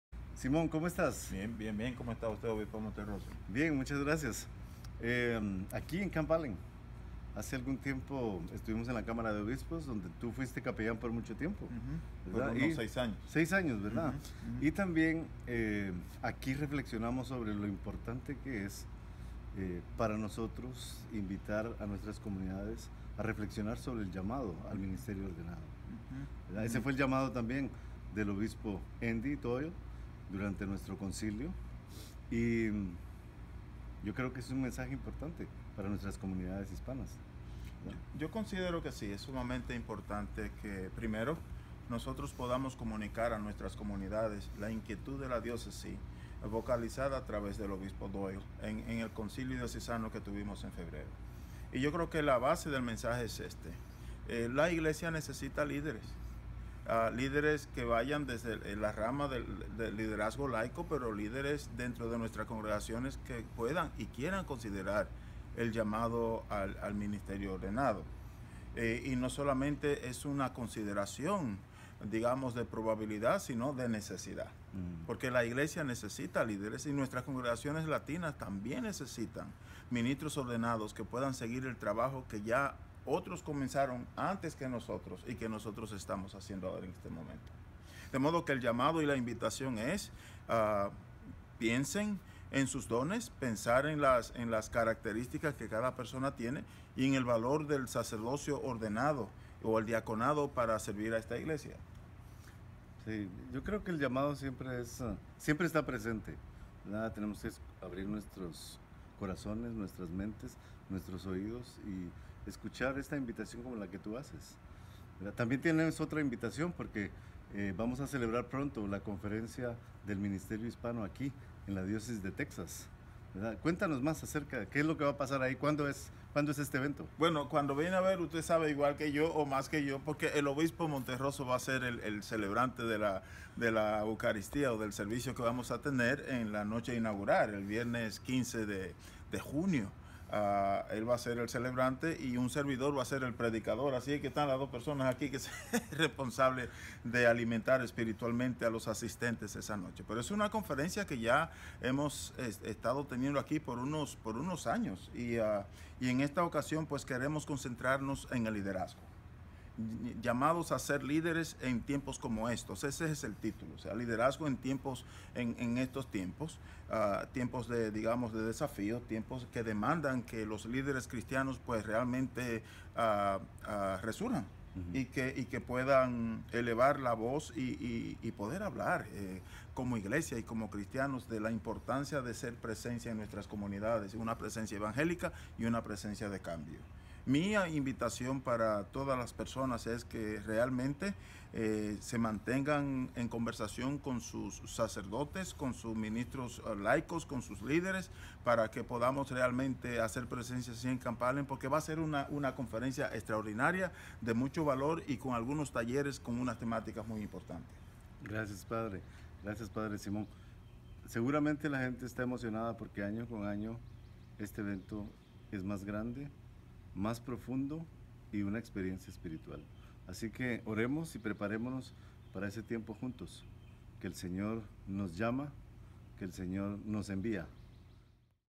El Rdo. Canónigo Simón Bautista da detalles de la próxima conferencia de líderes laicos
"La iglesia necesita líderes, líderes de la rama laico, líderes que puedan y quieran al llamado del ministerio ordenado", - El Rdo. Canónigo Simón Bautista de la Iglesia Catedral de Cristo, Houston.